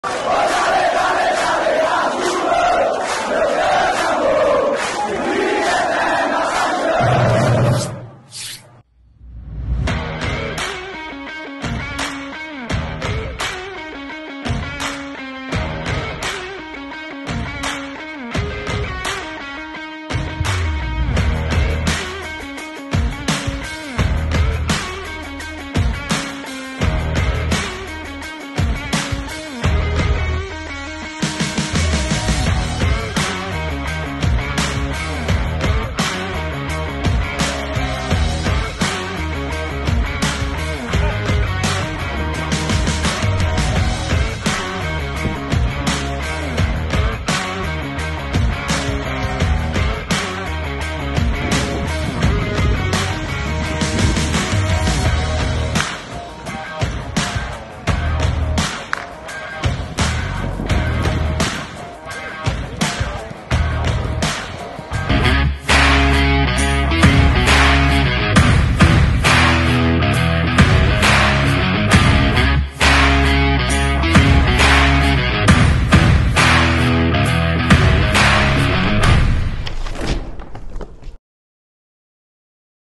Boletim de treino - 04.06.2021 | CSA TV
Amanhã tem Azulão. ⚽️👊🏼 Confira imagens do último trabalho antes da partida contra o Sampaio Corrêa, pela segunda rodada da @brasileiraob. 🔵⚪️

Seja Sócio do Maior e contribua com o nosso Azulão cada vez mais forte. Acesse o site e conheça os planos